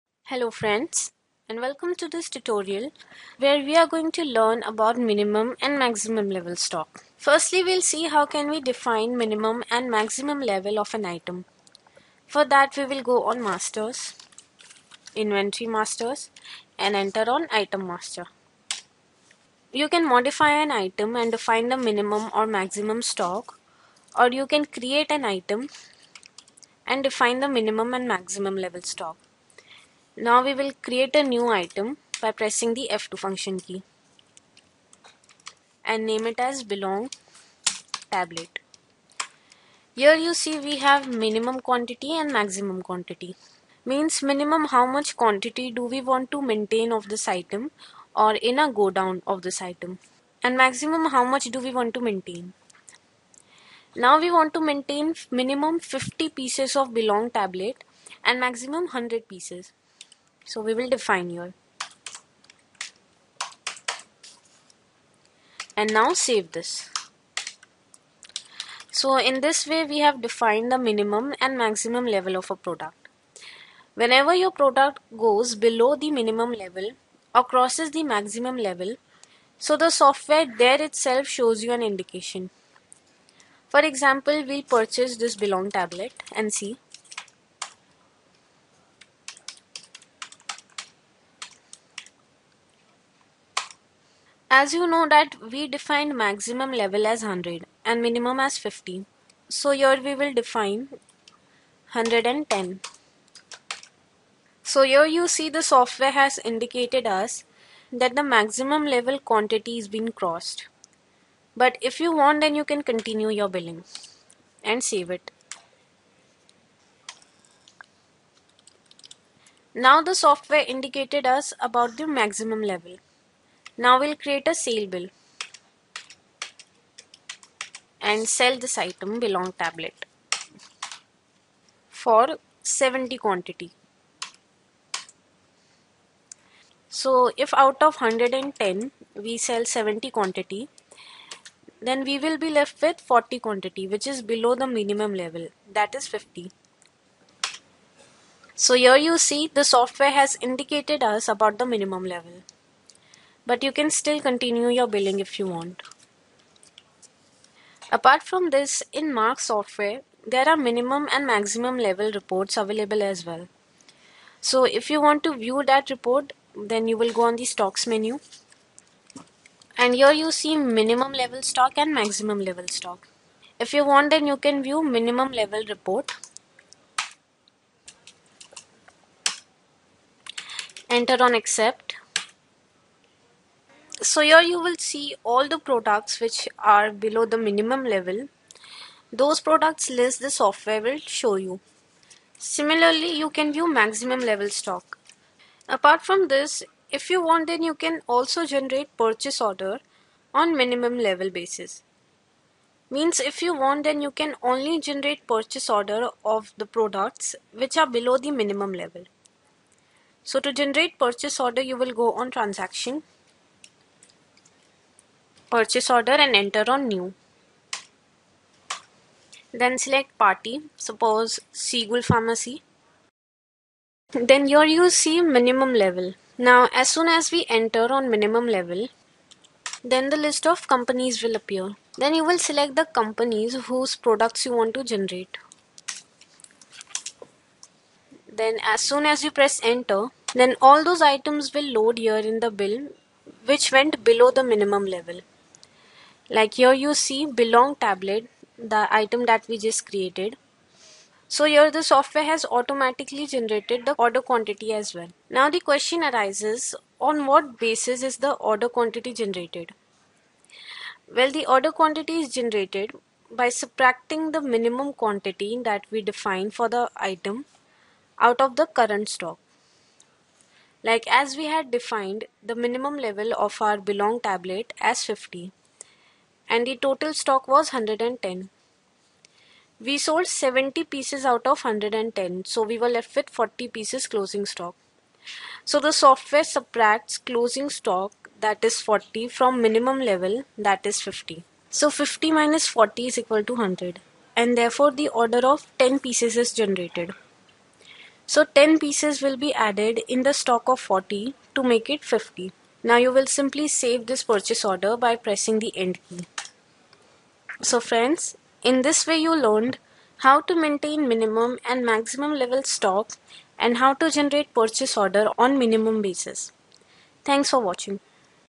Manage Minimum Maximum Level Stock [English]
The user maintains the Maximum and Minimum level stock as there are some products which are sold out in a defined quantity like when the sale of a XYZ product is less in any area. Then the retailer/distributor can manage the level of their stock.

The minimum and maximum level is maintained in the Item Master where the user can define the minimum and maximum quantity of the products.